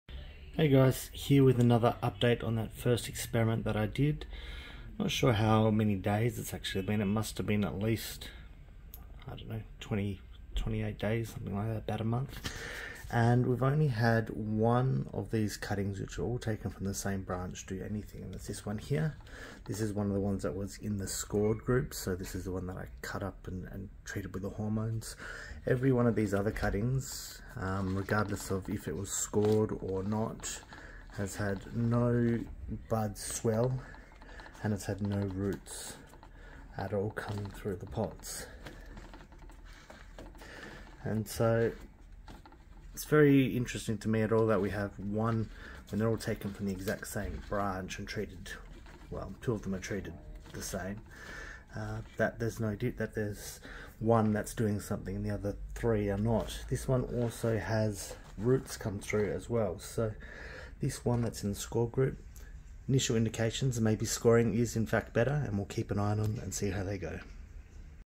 Fig cutting experiment 1 - Scored vs Unscored cuttings - First roots and buds appear
Out of the four plants in two groups, two cuttings were scored, two were left unscored and all were treated with rooting hormone.

Out of the four plants only one has done anything, and has put on roots and the beginnings of small leaves.

These cuttings were all off the same branch and are largely identical.